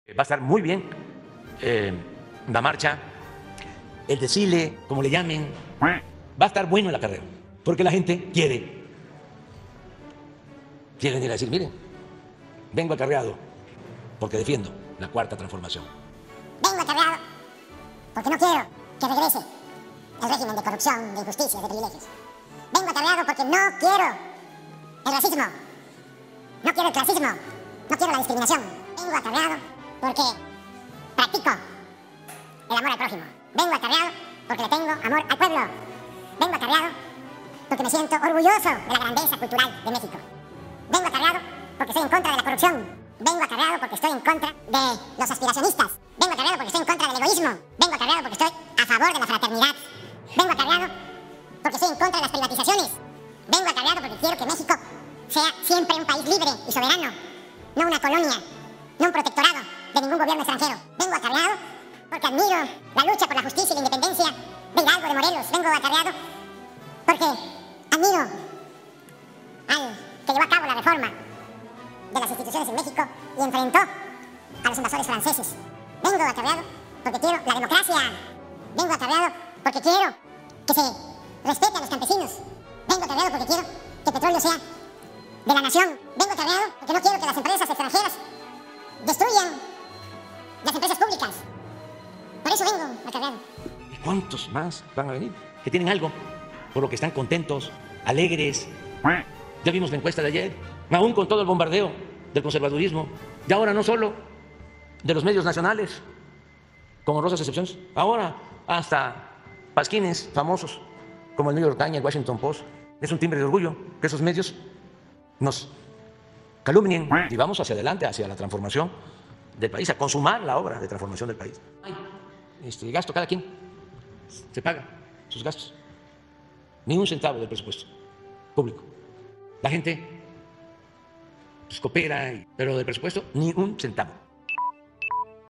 Sugiere que hagan cartulinas de “vengo acarreado porque…”, no costará un peso del presupuesto 🦆
Sugiere que hagan cartulinas “vengo acarreado porque defiendo la 4T”, “vengo acarreado porque estoy en contra de la corrupción”, “vengo acarreando porque estoy en contra de los aspiracionistas “ 🦆, etc. Enfatizó que no costará un peso del presupuesto público la MarchaDelEgo 🦆